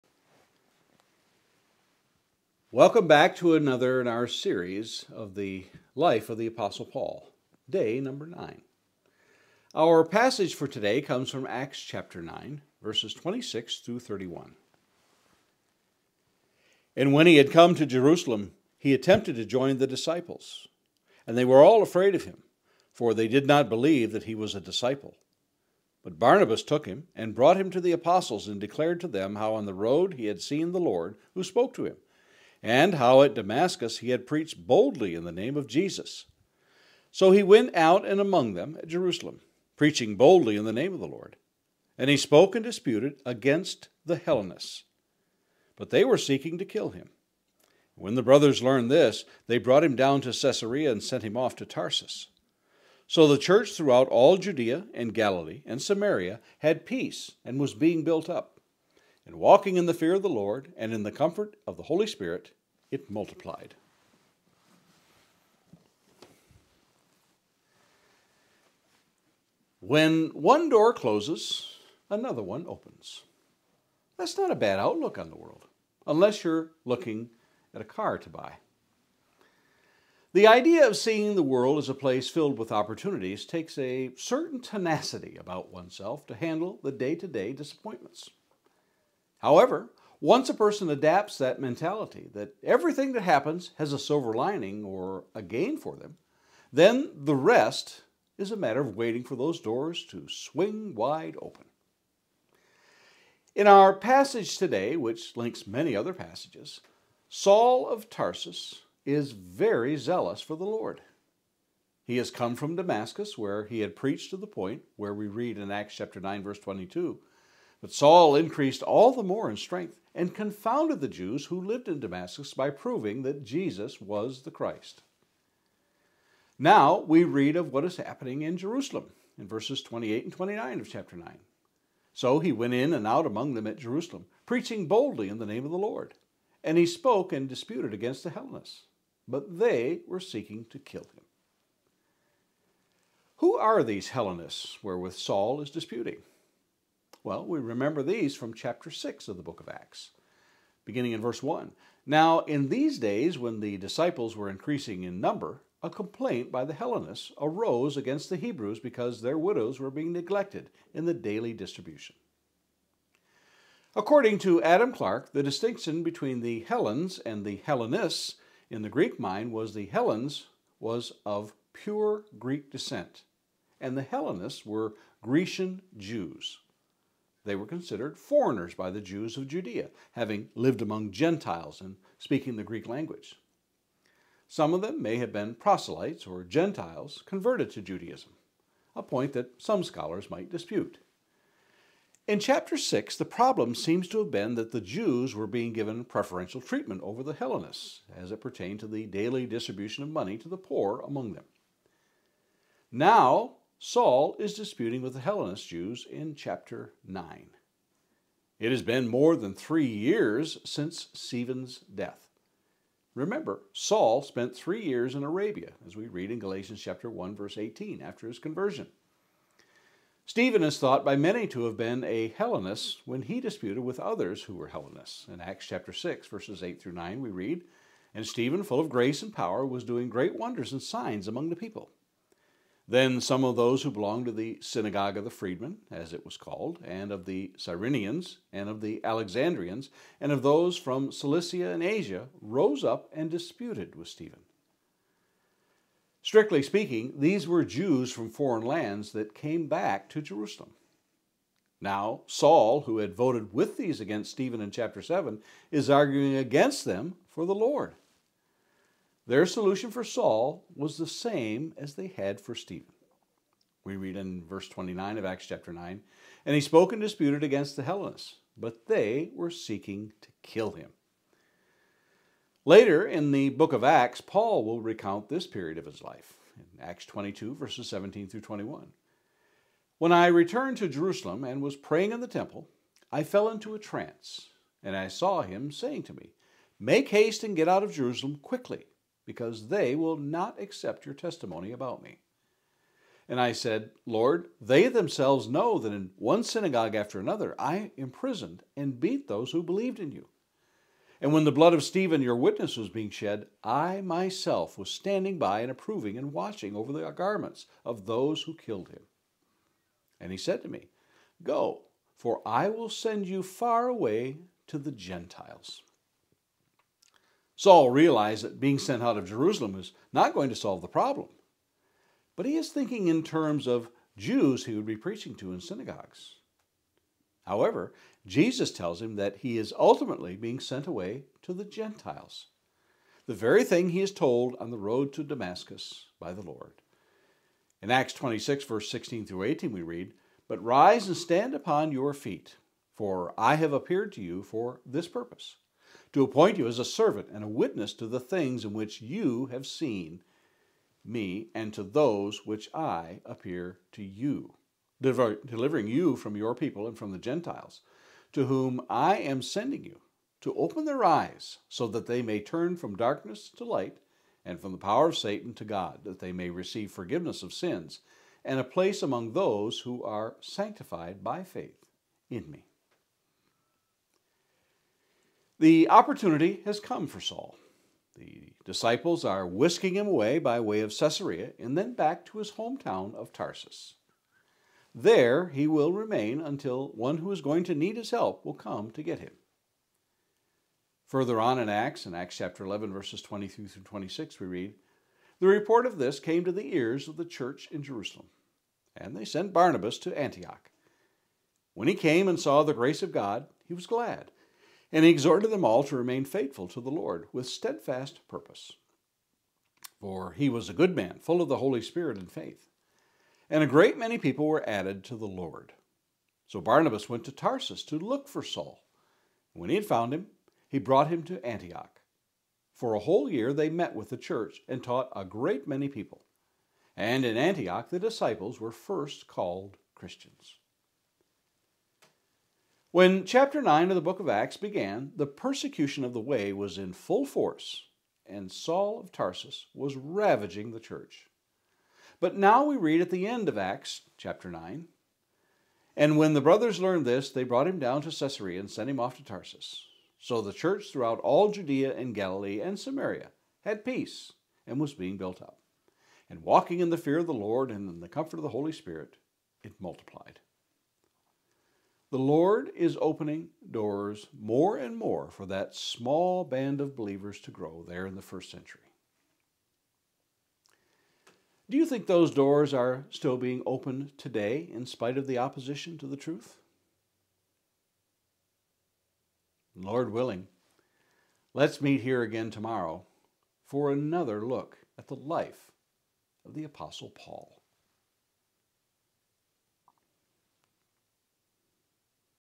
Day 9 The Life of the Apostle Paul: Saul Returns to Tarsus
A month-long daily devotional study in the life of the apostle Paul from the Book of Acts and his New Testament epistles. From his conversion to his last letters to Timothy.